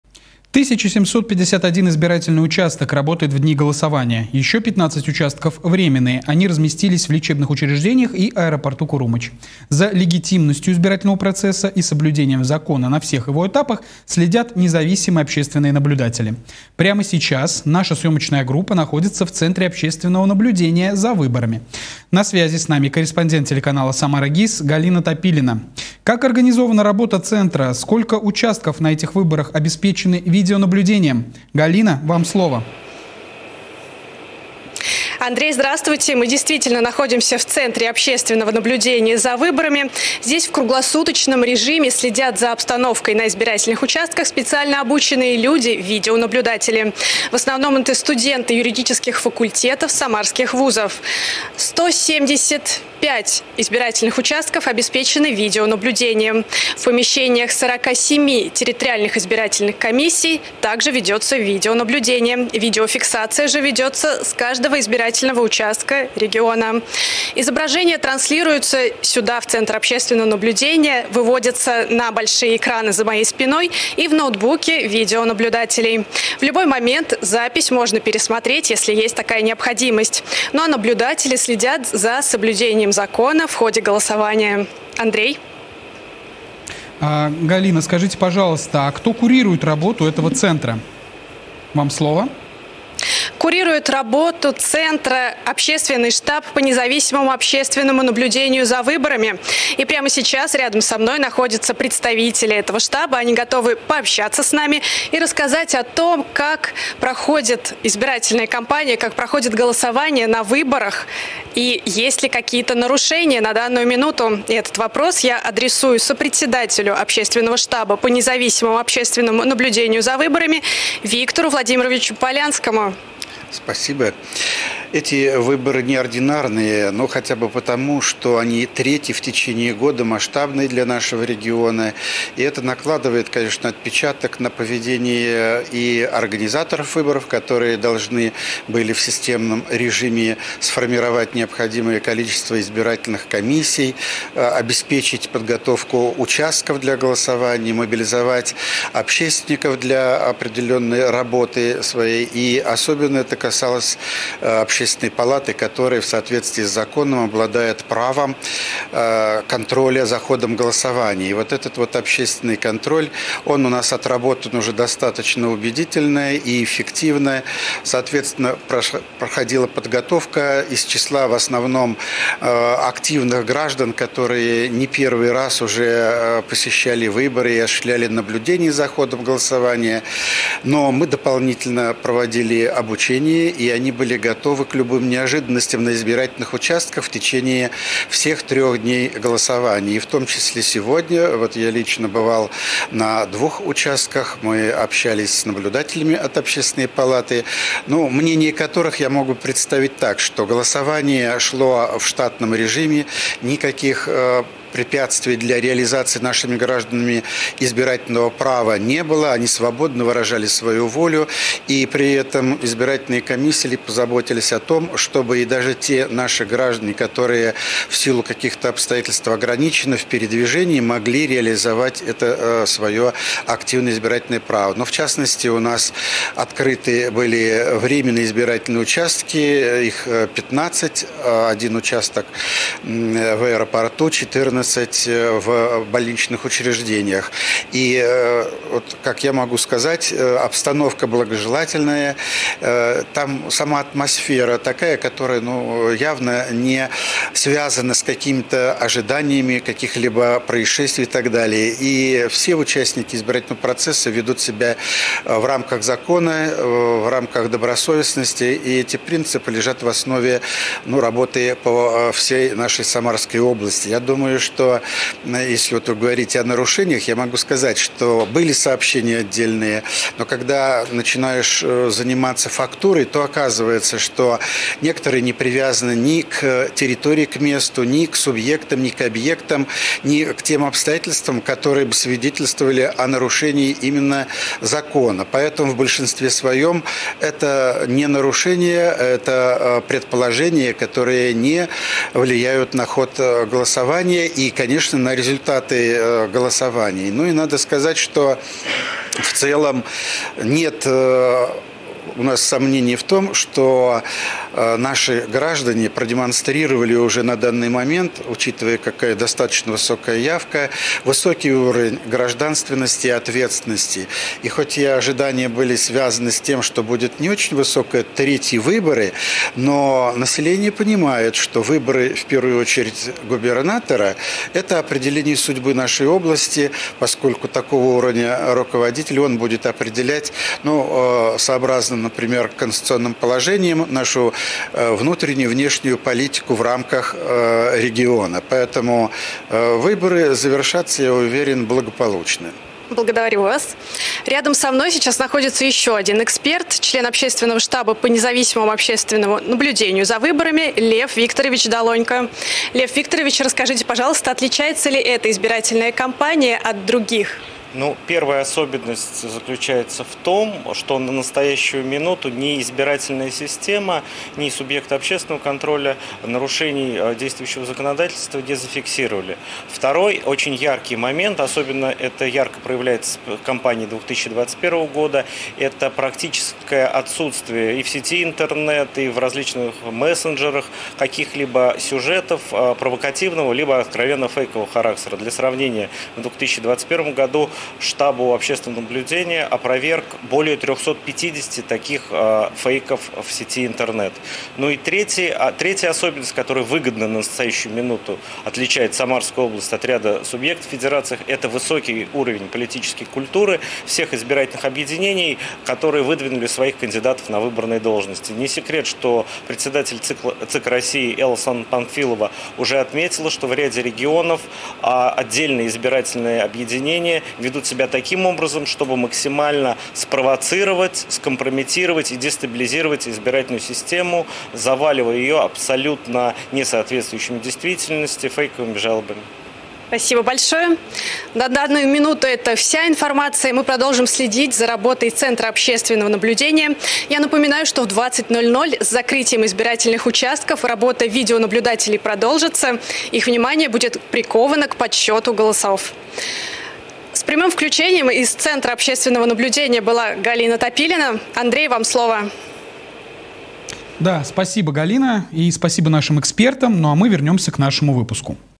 Наблюдатели следят за третьим днем голосования в регионе
1751 избирательный участок работает в дни голосования, еще 15 участков - временные, они разместились в лечебных учреждениях и аэропорту Курумоч. За легитимностью избирательного процесса и соблюдением закона на всех его этапах следят независимые общественные наблюдатели.